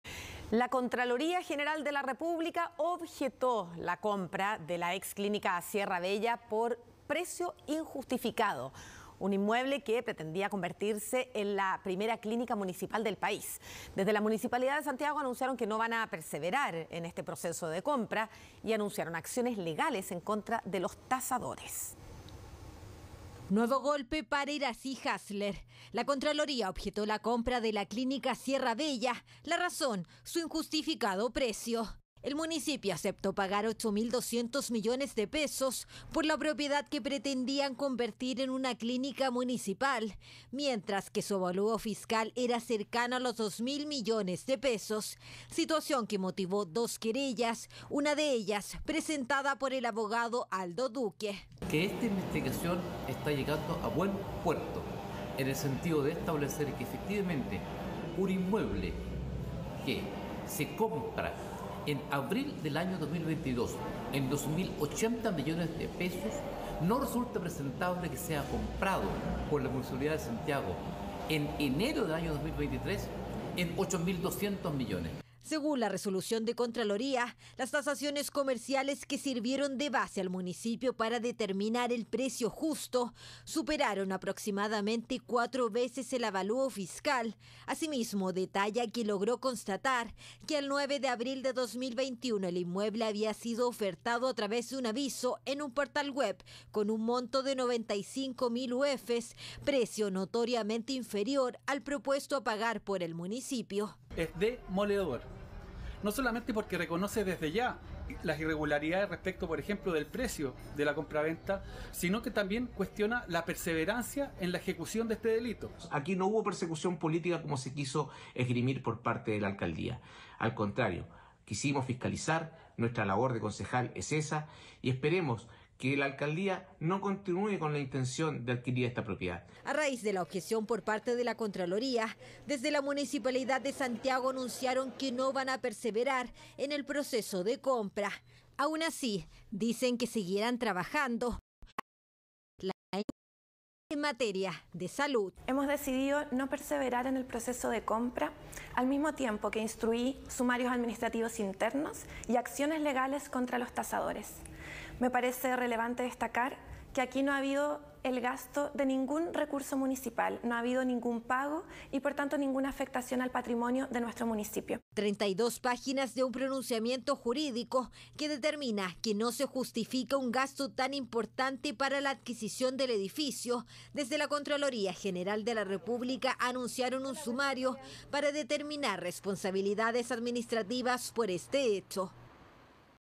Contraloría objetó compra de Clínica Sierra Bella por parte de la Municipalidad de Santiago
El organismo constató que el municipio liderado por Irací Hassler adquirió el establecimiento por un precio cuatro veces mayor al valor por el que fue previamente adquirido por la vendedora.